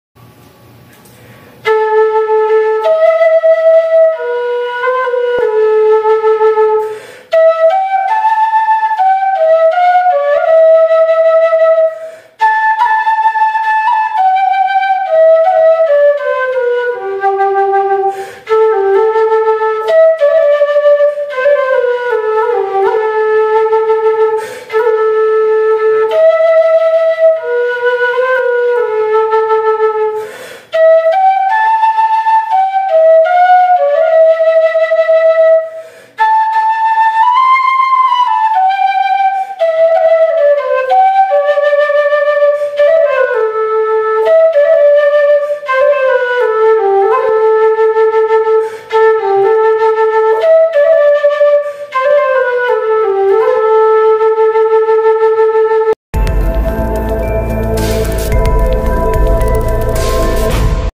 Quena de bambu - G - 440 Hz
Quena feita em bambu, afinada em G (Sol) - 440 Hz.